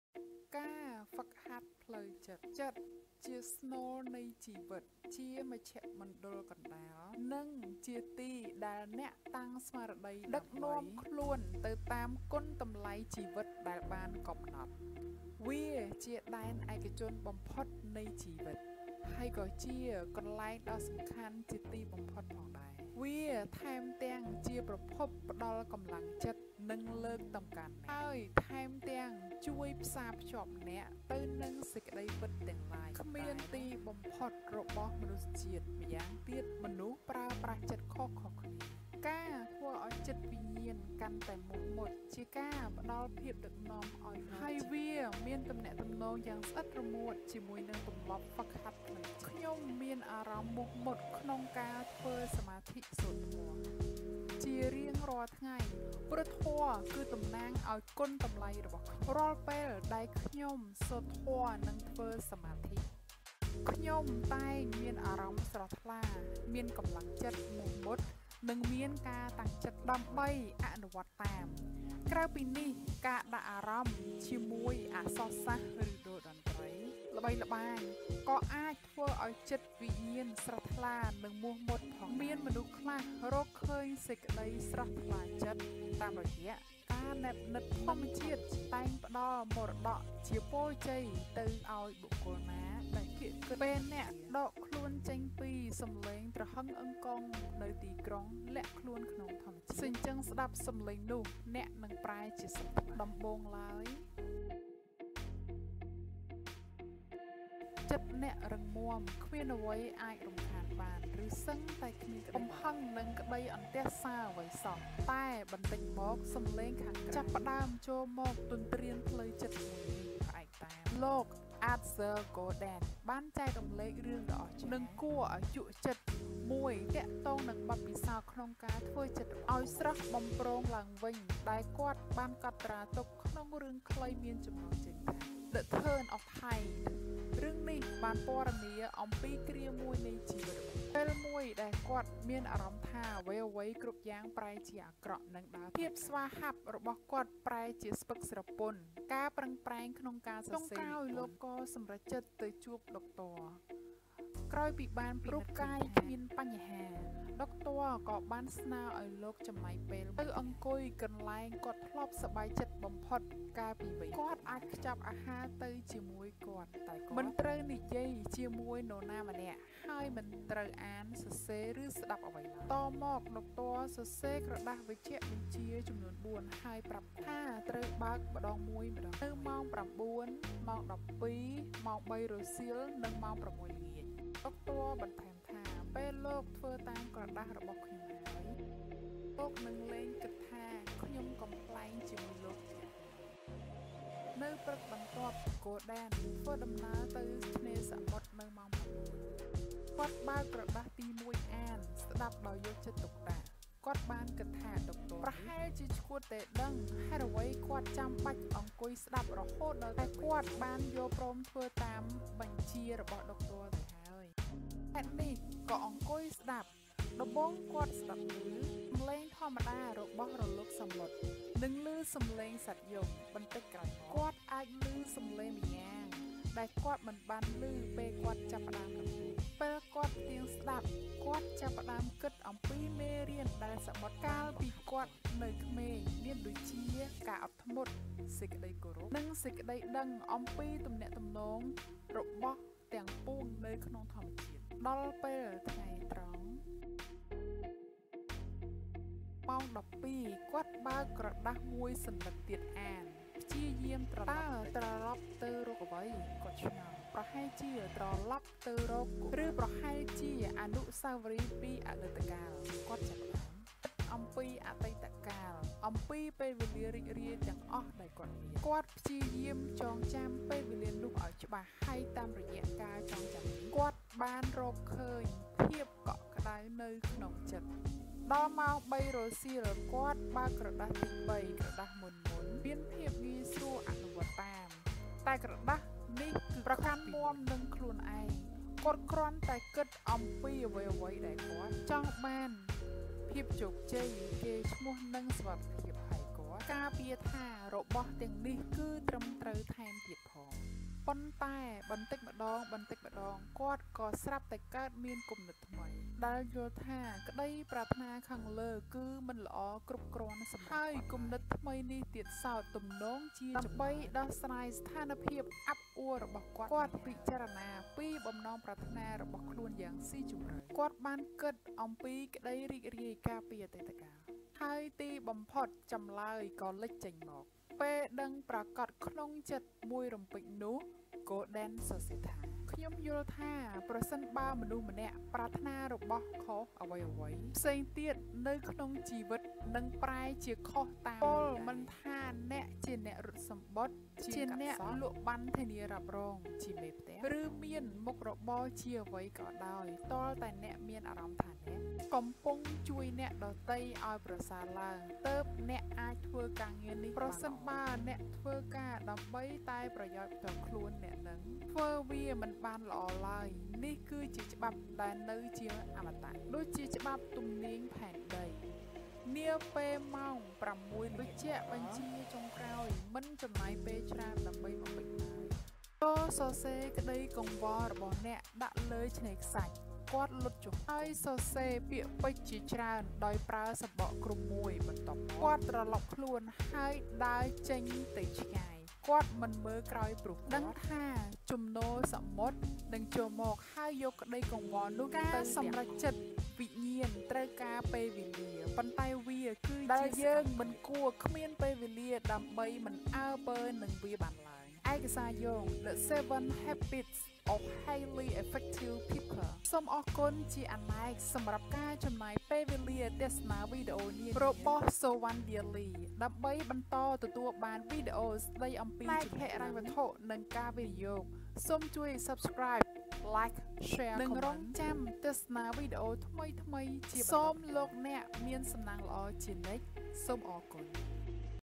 Trips# Sovann Daily#MentalTraining
ចិត្ត ជាស្នូលនៃជីវិត ជាមជ្ឈមណ្ឌលកណ្តាល និងជាទីដែលអ្នកតាំងស្មារតី ដើម្បីដឹកនំាខ្លួនទៅតាមគុណតម្លៃជីវិតដែលបានកំណត់។ វាជាដែនឯកជនបំផុតនៃជីវិត ហើយក៏ជាកន្លែងដ៏សំខាន់ជាទីបំផុតផងដែរ។  វាថែមទាំងជាប្រភពផ្តល់កម្លាំងចិត្ត និងលើកតម្កើនអ្នកហើយថែមទំាងជួយផ្សារភ្ជាប់អ្នកទៅនឹងសេចក្តីពិតទំាងឡាយ ដែលគ្មានទីបំផុតរបស់មនុស្សជាតិ ម្យ៉ាវិញទៀតមនុស្សប្រើប្រាស់ចិត្តខុសៗគ្នា(Stephen R. Convey, 2017)។
ការធ្វើឱ្យចិត្តវិញាណកាន់តែមោះមុត ជាការផ្តល់ភាពដឹកនាំឱ្យដល់ជីវិតអ្នក ហើយវាមានទំនាក់ទំនងយ៉ាងស្អិតរមួតជាមួយនឹងទម្លាប់ហ្វឹកហាត់ផ្លូវចិត្ត។
ខ្ញុំមានអារម្មណ៍មោះមុតនឹងការធ្វើសមាធិសូត្រធម៌ជារៀងរាល់ថ្ងៃ ព្រោះធម៌គឺតំណាងឱ្យគុណតម្លៃរបស់ខ្ញុំ។ រាល់ពេលដែលខ្ញុំអានធម៌ និងធ្វើសមាធិ  ខ្ញុំតែងមានអារម្មណ៍ស្រស់ថ្លា មានកម្លាំងចិត្ត មោះមុត និងមានការតំាងចិត្តឡើងវិញ ដើម្បីអនុវត្តតាម។ ក្រៅពីនេះ ការដាក់អារម្មណ៍ជាមួយអក្សរសាស្រ្ត ឬដូរតន្ត្រីល្បីល្បាញ ក៏អាចធ្វើឱ្យចិត្តវិញាណស្រស់ថ្លា និងមោះមុតផងដែរ។ មានមនុស្សខ្លះរកឃើញសេចក្តីស្រស់ថ្លាចិត្ត តាមរយៈការនែបនិត្យនឹងធម្មជាតិ។ ធម្មជាតិតែងផ្តល់មរតកជាពរជ័យទៅឱ្យបុគ្គលណាដែលគៀកគិតនឹងវា។ វេលាពេលអ្នកអាចដកខ្លួនចេញអំពីសម្លេងត្រហឹងអើងកងនៅទីក្រុង ហើយលាក់ខ្លួននៅក្នុងធម្មជាតិ រួចសញ្ជឹងស្តាប់សម្លេងរបស់វា អ្នកនឹងប្រែជាស្រស់ថ្លាឡើងវិញមិនខាន។ ជាដំបូងឡើយ ចិត្តរបស់អ្នករឹងមំាំ គ្មានអ្វីអាចរំខានបាន ឬសឹងតែគ្មានក្តីបារម្ភ កំហឹង និងក្តីអន្ទះសារអ្វីសោះ ប៉ុន្តែបន្តិចម្តងៗ សម្លេងពីពិភពខាងក្រៅ ចាប់ផ្តើមចូលមកទន្ទ្រានផ្លូវចិត្តមួយនេះ (Stephen R. Convey, 2017)។
ផ្អែកតាម លោក អាហ្ស៊ ហ្គោឌិន(Artur Gordon) បានចែករំលែករឿងដ៏អាស្ចារ្យ និងគួរឱ្យជក់ចិត្តមួយទាក់ទងនឹងបទពិសោធន៍ក្នុងការធ្វើចិត្តរបស់គាត់ឱ្យស្រស់បំព្រងឡើងវិញ  ហើយដែលគាត់បានកត់ត្រាទុកនៅក្នុងរឿងខ្លី មានចំណងជើថា “The Turn of the Tide” ។ រឿងនេះបានពណ៌នាអំពីគ្រាមួយនៃជីវិតរបស់គាត់ នៅពេលមួយដែលគាត់ចាប់ផ្តើមមានអារម្មណ៍ថាអ្វីៗគ្រប់យ៉ាងប្រែជាអាក្រក់ និងដើរថយក្រោយ។ ភាពស្វាហាប់របស់គាត់ប្រែជាស្ពឹកស្រពន់ ការប្រឹងប្រែងក្នុងការសរសេរនិពន្ធបានប្រែជាគ្មានបានលទ្ធផល ហើយស្ថានភាពវិវត្តន៍កាន់តែអាក្រក់ទៅពីថ្ងៃទៅមួយថ្ងៃ។ ចុងក្រោយលោកក៏សម្រេចចិត្តទៅជួបដុកទ័រ។ ក្រោយពីបានពិនិត្យឃើញថារូបកាយគ្មានបញ្ហាអ្វីកើតឡើង ដុកទ័រក៏បានស្នើឱ្យលោកចំណាយពេលមួយថ្ងៃ (3:44.07 , 3.77) ទៅអង្គុយកន្លែងដែលគាត់ធ្លាប់សប្បាយចិត្តបំផុតកាលពីនៅវ័យកុមារ ។ គាត់អាចខ្ចប់អាហារទៅជាមួយ តែគាត់មិនត្រូវនិយាយជាមួយនរណាម្នាក់ ហើយមិនត្រូវអាន សរសេរ ឬស្តាប់អ្វីឡើយ។ តមកដុកទ័រសរសេរក្រដាសវេជ្ជបញ្ជាចំនួនបួន ហើយប្រាប់គាត់ថាត្រូវបើកម្តងមួយៗ នៅម៉ោង ៩, ម៉ោង ១២, ម៉ោង ០៣រសៀល និងម៉ោង៦ ល្ងាច។ ដុក៧ទ័របន្ថែមថា “ពេលលោកធ្វើតាមក្រដាសរបស់ខ្ញុំហើយ លោកនឹងលែងគិតថាខ្ញុំបំប្លែងជាមួយលោកទៀតទេ ” (Stephen R. Convey, 2017)។
នៅព្រឹកបន្ទាប់ ហ្គោដិន(Gordon) ធ្វើដំណើរទៅឆ្នេរសមុទ្រ។ នៅពេលម៉ោង ៩ គាត់បើកក្រដាសទី១ អាន “ស្តាប់ដោយយកចិត្តទុកដាក់”។ គាត់បានគិតថាដុកទ័រនេះប្រហែលជាឆ្កួតទេដឹង ។ ហេតុអ្វីគាត់ចំាបាច់ត្រូវអង្គុយស្តាប់រហូតដល់ទៅបីម៉ោង? តែគាត់បានយល់ព្រមធ្វើតាមការបញ្ជារបស់ដុកទ័រទៅហើយ  ហេតុនេះក៏អង្គុយស្តាប់។ ដំបូងគាត់ស្តាប់ ឮសម្លេងធម្មតារបស់រលកសមុទ្រ និងសម្លេងសត្វយំ។ បន្តិចក្រោយមក គាត់អាចឮសម្លេងម្យ៉ាងដែលគាត់មិនបានឮពេលគាត់ចាប់ផ្តើមដំបូង។ ពេលគាត់ផ្ទៀងស្តាប់ គាត់ចាប់ផ្តើមគិតអំពីមេរៀនដែលសមុទ្របានបង្ហាត់គាត់កាលពីគាត់នៅក្មេង មានដូចជាការអត់ធ្មត់  សេចក្តីគោរព និងសេចក្តីដឹងអំពីទំនាក់ទំនងនៃរបស់ទំាងពួងនៅក្នុងធម្មជាតិ។ គាត់ចាប់ផ្តើមស្តាប់សម្លេងទំាងនោះដោយស្ងៀមស្ងាត់ កាន់តែស្តាប់កាន់តែមានអារម្មណ៍សន្តិភាព(Stephen R. Convey, 2017)។
ដល់ពេលថ្ងៃត្រង់ ម៉ោង ១២ គាត់បើកក្រដាសមួយសន្លឹកទៀតអាន “ព្យាយាមត្រឡប់ទៅក្រោយ”។ “តើត្រឡប់ទៅរកអ្វី?”គាត់ឆ្ងល់។ ប្រហែលជាត្រឡប់ទៅរកកុមារភាព ឬប្រហែលជាអនុស្សាវរីយពីអតីតកាល។ គាត់ចាប់ផ្តើមគិតអំពីអតីតកាល អំពីពេលវេលារីករាយទំាងអស់ដែលគាត់មាន។ គាត់ព្យាយាមចងចំាពេលវេលានោះឱ្យច្បាស់ ហើយតាមរយៈការចងចំានេះ គាត់បានរកឃើញភាពកក់ក្តៅនៅខាងក្នុងចិត្ត(Stephen R. Convey, 2017)។
ដល់ម៉ោងបីរសៀល គាត់បានបើកក្រដាសទី៣ ។ ក្រដាសមុនៗមានភាពងាយស្រួលក្នុងការអនុវត្តតាម ក៏ប៉ុន្តែក្រដាសមួយនេះគឺខុសពីមុន។ វានិយាយថា “ពិនិត្យបំណងប្រាថ្នារបស់អ្នក”។ មុនដំបូងគាត់ប្រកាន់មាំនឹងខ្លួនឯង។ គាត់គ្រាន់តែគិតអំពីអ្វីៗ ដែលគាត់ចង់បានដូចជា ភាពជោគជ័យ កេរ្តិ៍ឈ្មោះ និងសុវត្ថិភាពជាដើម ហើយគាត់ការពារថារបស់ទំាងនេះ គឺត្រឹមត្រូវថែមទៀតផង។  ប៉ុន្តែបន្តិចម្តងៗគាត់ក៏ស្រាប់តែកើតមានគំនិតថ្មីដែលយល់ថាក្តីប្រាថ្នាខាងលើ គឺមិនល្អគ្រប់គ្រាន់សម្រាប់គាត់ឡើយ ហើយគំនិតថ្មីនេះទៀតសោត ទម្នងជាចម្លើយ ដើម្បីដោះស្រាយស្ថានភាពអាប់អួររបស់គាត់នេះហើយ(Stephen R. Convey, 2017)។ (7: 50.86)
គាត់ពិចារណាពីបំណងប្រាថ្នារបស់ខ្លួនយ៉ាងស៊ីជម្រៅ។ គាត់បានគិតអំពីក្តីរីករាយកាលពីអតីតកាល ហើយទីបំផុត ចម្លើយក៏លេចចេញមក។ “ពេលដឹងប្រាកដក្នុងចិត្តមួយរំពេចនោះ” ហ្គោដិន (Gordon) សរសេរថា “ខ្ញុំក៏យល់ថាប្រសិនបើមនុស្សម្នាក់ប្រាថ្នារបស់ខុស អ្វីៗផ្សេងទៀតនៅក្នុងជីវិតនឹងប្រែជាខុសទៅតាមនោះដែរ”។ ពោល មិនថាអ្នកជាអ្នករត់សំបុត្រ ជាងកាត់សក់  ជាអ្នកលក់ប័ណ្ណធានារ៉ាប់រង ជាមេផ្ទះ ឬមានមុខរបរជាអ្វីក៏ដោយ។ ទាល់តែអ្នកមានអារម្មណ៍ថាអ្នកកំពុងជួយអ្នកដទៃឱ្យប្រសើរឡើងទើបអ្នកអាចធ្វើការងារនេះបានល្អ ។ ប្រសិនបើអ្នកធ្វើការដើម្បីតែប្រយោជន៍ផ្ទាល់ខ្លួនឯង អ្នកនឹងធ្វើវាមិនបានល្អឡើយ នេះគឺជាច្បាប់ដែលនៅជាអមតៈដូចជាច្បាប់ទំនាញផែនដី(Stephen R. Convey, 2017)។
នាពេលម៉ោងប្រាំមួយល្ងាចមកដល់ វេជ្ជបញ្ជាចុងក្រោយមិនចំណាយពេលច្រើនដើម្បីបំពេញឡើយ។ “ចូរសរសេរក្តីកង្វល់របស់អ្នកដាក់លើឆ្នេរខ្សាច់”។ គាត់លុតជង្គង់ ហើយសរសេរពាក្យពេចន៍ជាច្រើនដោយប្រើសំបកគ្រុំមួយ បន្ទាប់មកគាត់ត្រឡប់ខ្លួនហើយដើរចេញទៅឆ្ងាយ។ គាត់មិនមើលក្រោយព្រោះគាត់ដឹងថាជំនោរសមុទ្រនឹងចូលមក ហើយយកក្តីកង្វល់នោះទៅទំាងអស់។ ការសម្រួចចិត្តវិញាណត្រូវការពេលវេលា ប៉ុន្តែវាគឺជាសកម្មភាពដែលយើងមិនគួរគ្មានពេលវេលា ដើម្បីមិនអើពើនឹងវាបានឡើយ(Stephen R. Convey, 2017)។